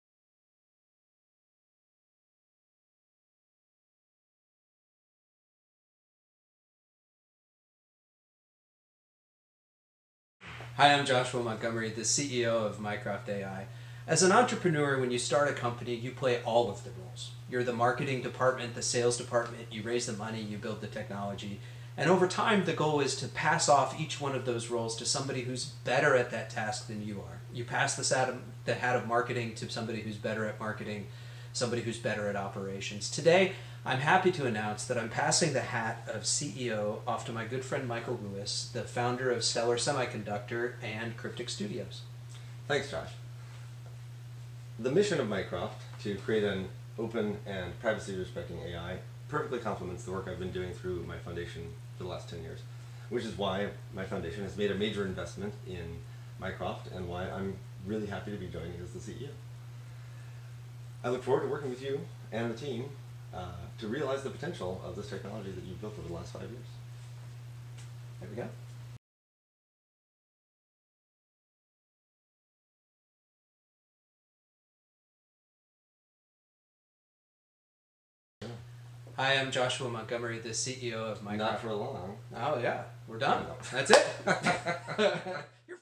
Michael Lewis Joins Mycroft as CEO
We are excited to announce Michael Lewis, of Stellar Semiconductors, Cryptic Studios, and the Anspar Foundation, is joining Mycroft AI as CEO.

How can you get involved with Mycroft? Download it and join the conversation on our forum!